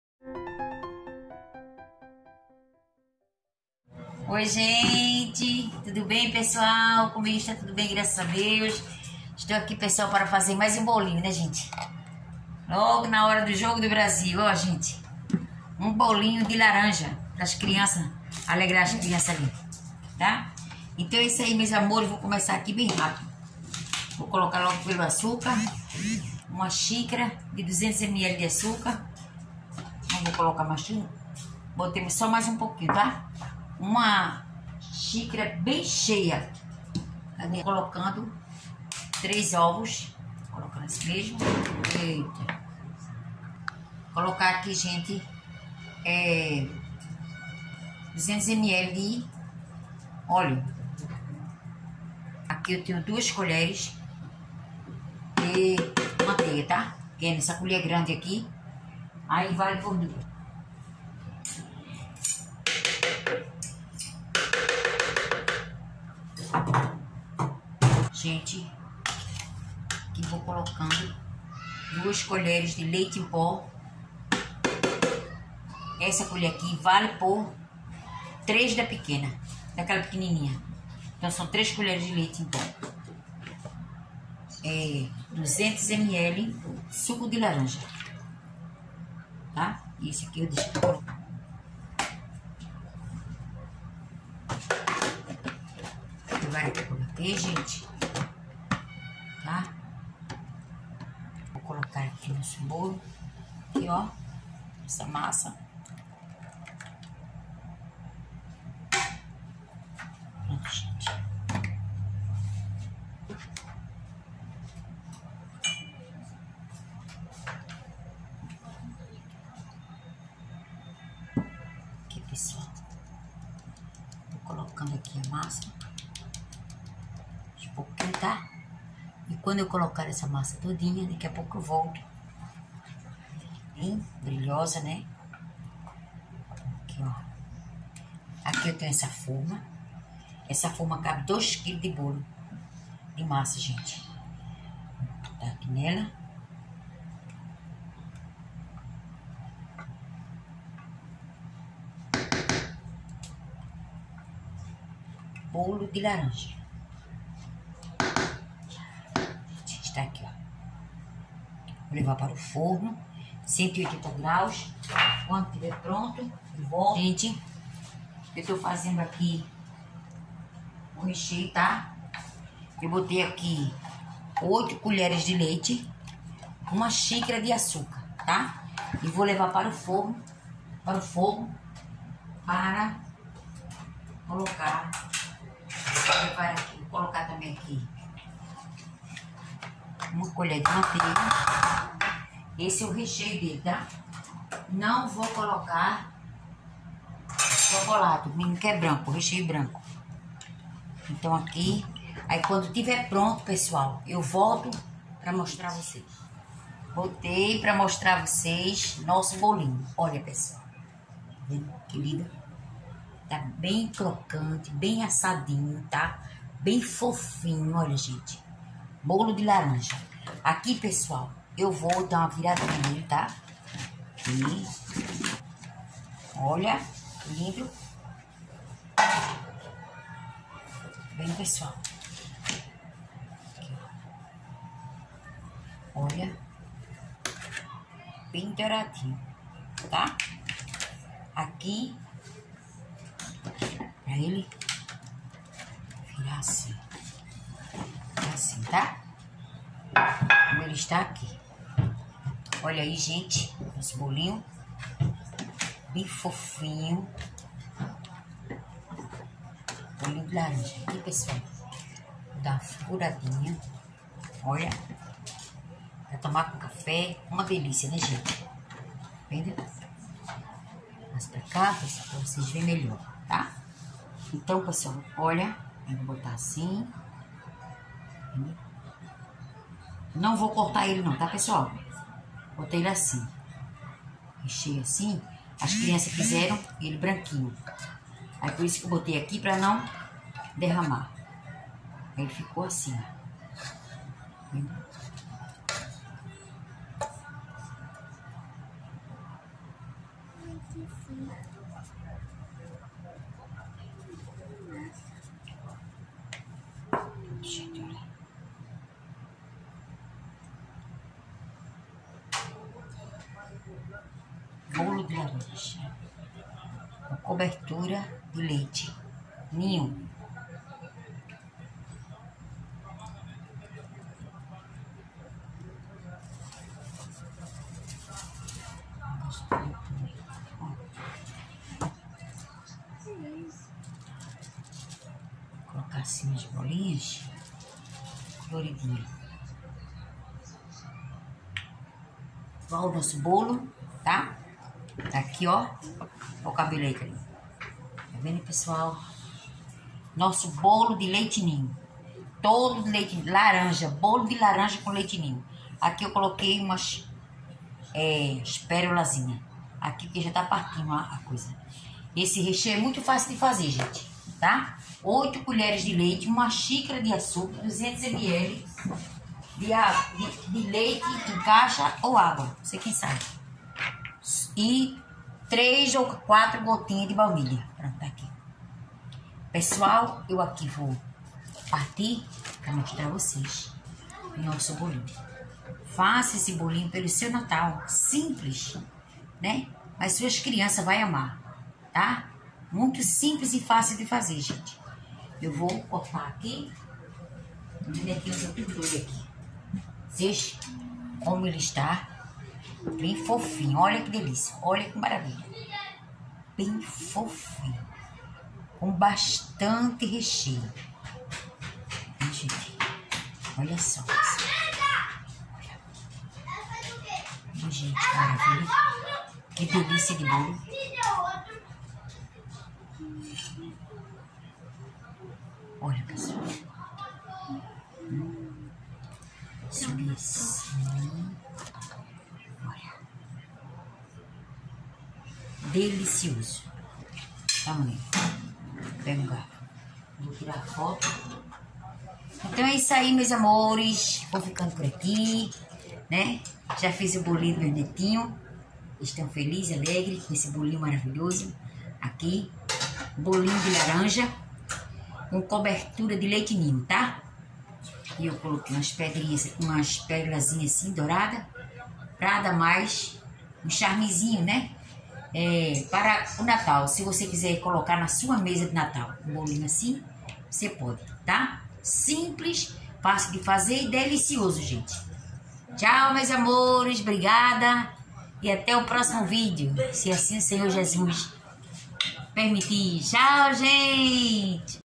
bolo de laranja com cobertura de leite ninho
4 xícara de trigo com fermento
250 grama de  açúcar
3 colheres de margarina
200 lm de óleo
300 ml de suco de laranja
3 gotinha de balnilha
3 ovos inteiro

cobertura
8 colheres leite ninho ou da sua preferência
1 colher de margarina
1 xícara de 200 ml de açúcar
3 gotinhas de balnilha leve tudo para ou fogo baixo até dar ou ponto deixe esfriar desenforme ou bolo depeije toda cobertura polvilhe bastante leite ninho decoração agosto